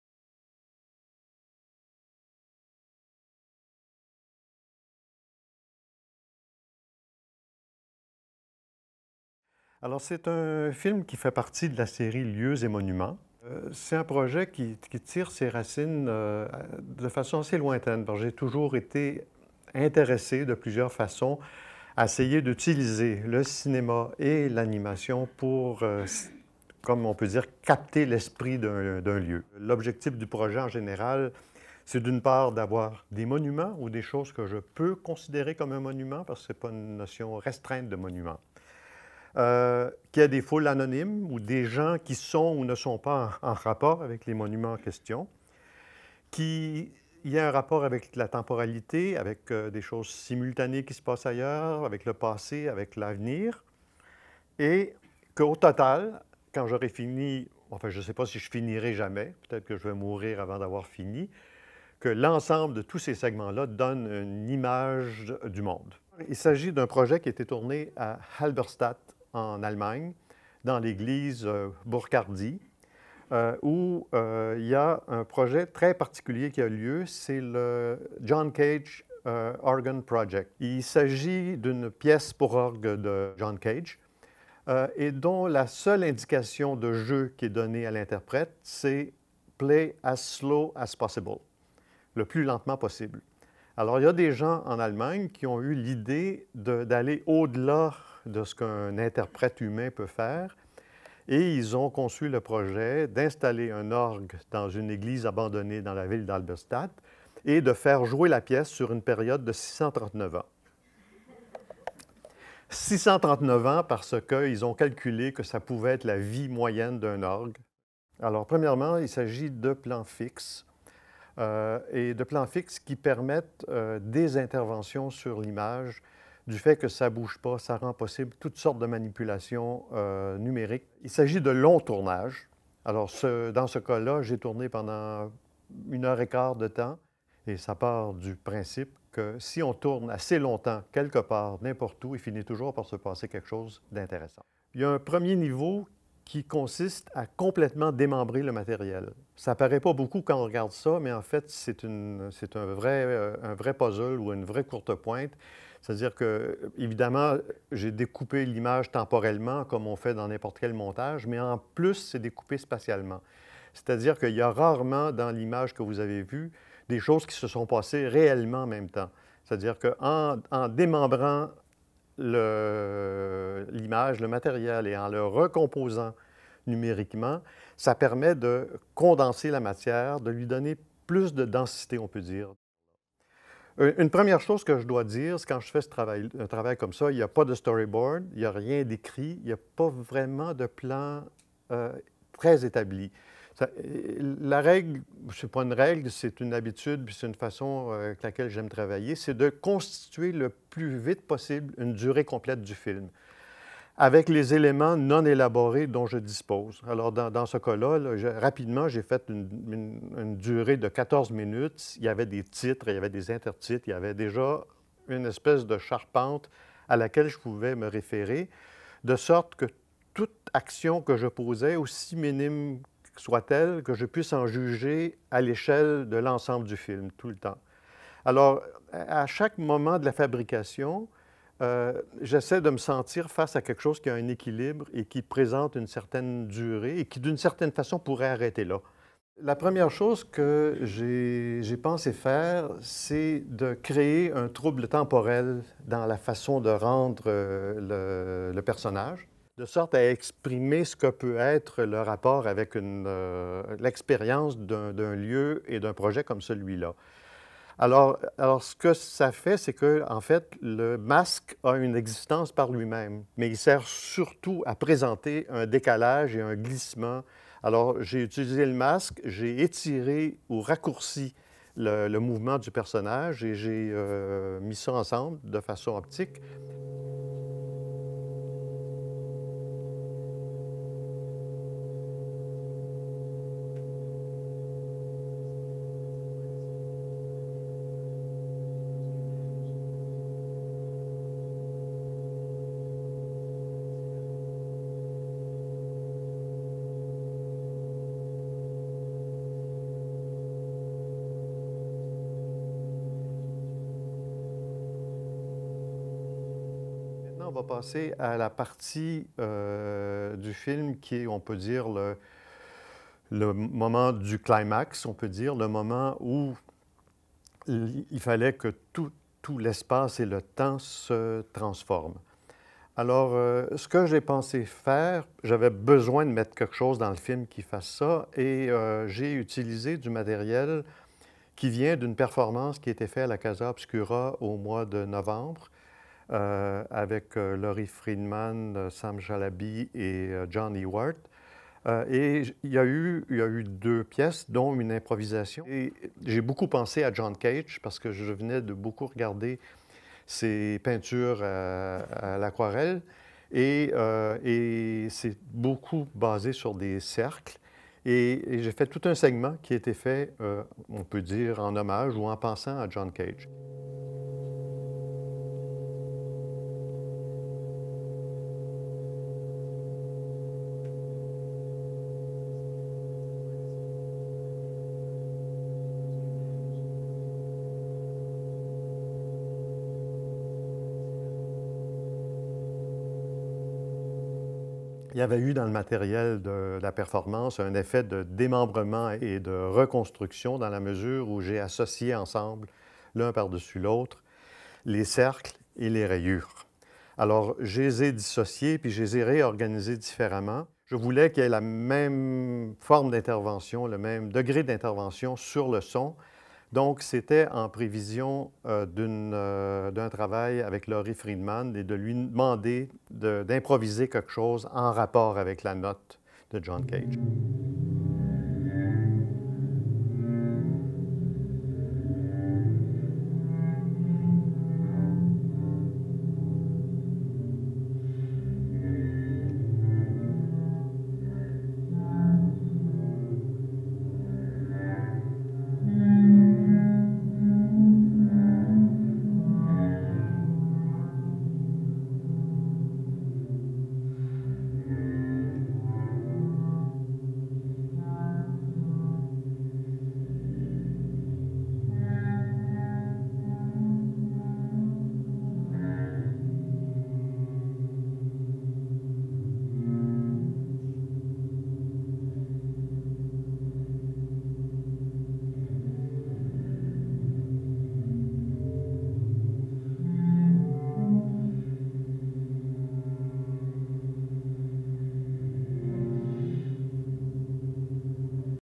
Classe de maître: Pierre Hébert
Extraits de la classe de maître du réalisateur Pierre Hébert du Jeudi 7 février 2013, à la Cinémathèque québécoise, dans le cadre de 5 sur 5 Animations.
La série 5 sur 5 animations est une coréalisation de la Cinémathèque
québécoise et de Vidéographe.
Suivez les commentaires, vidéos et leçons des réalisateurs sur la page
Facebook de 5 sur 5 animations.